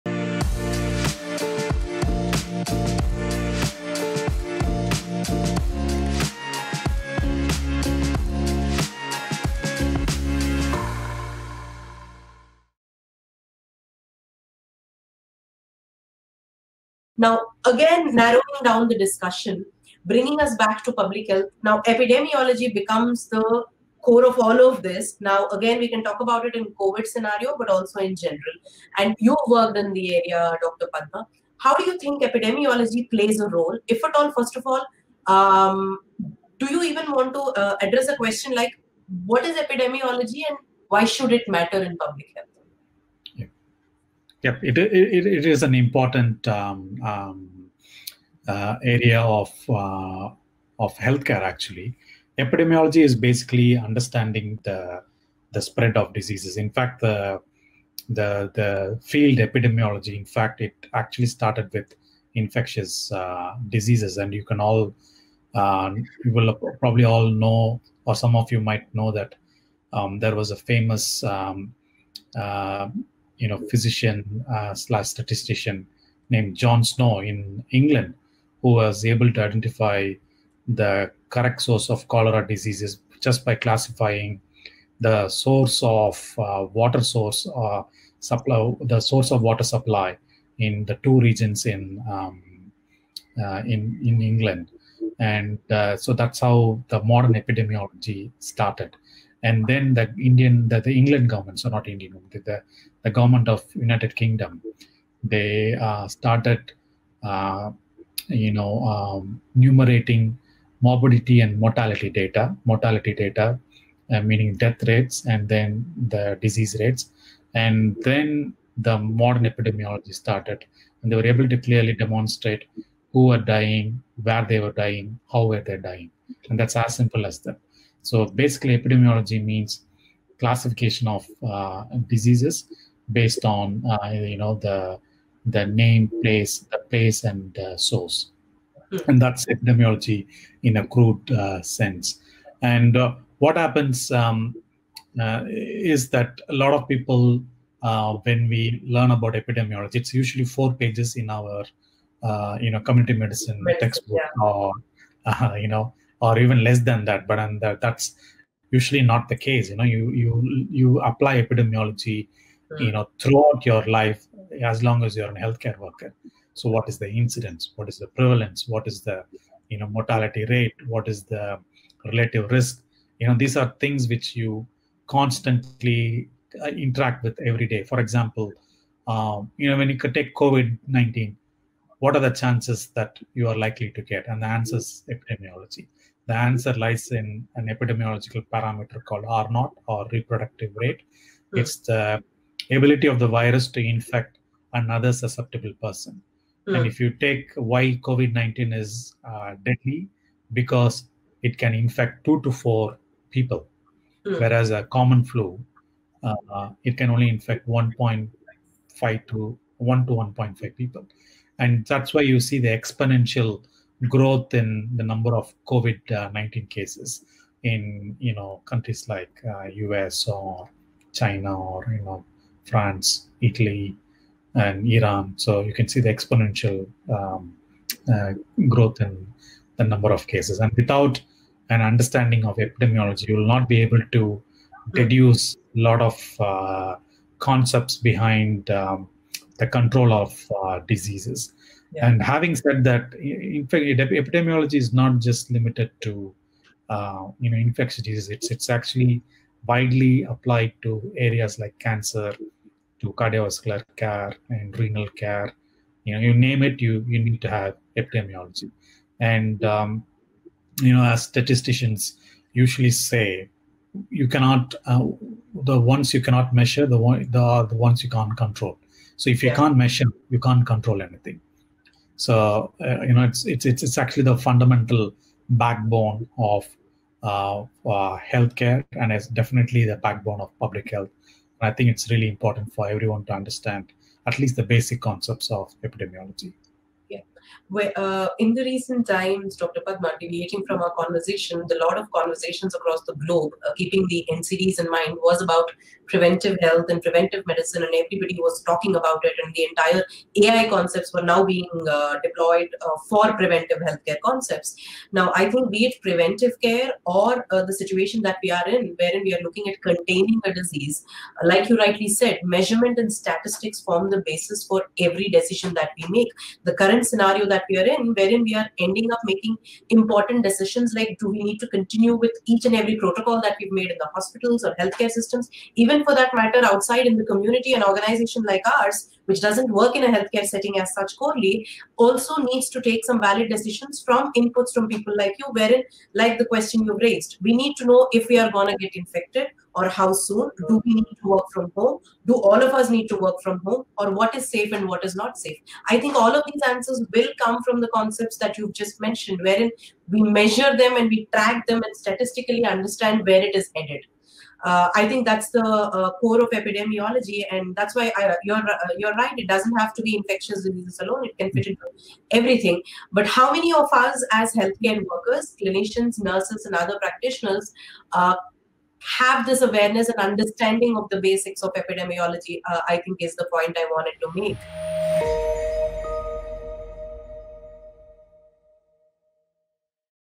Role of Epidemiology in Public Health
The study of the origins and causes of diseases in a community is known as epidemiology. It is the scientific method of investigating and solving problems used by disease detectives, epidemiologists, statisticians, and public health professionals to get to the bottom of community health problems and outbreaks.

Epidemiology is frequently regarded as the primary scientific foundation of public health practice. Here’s everything you need to know about epidemiological parameters in pandemic.

Dr. Padmanesan Narasimhan, Faculty and consultant, UNSW, discusses what epidemiology is? Why should it matter in public health? And the role of epidemiology in the pandemic.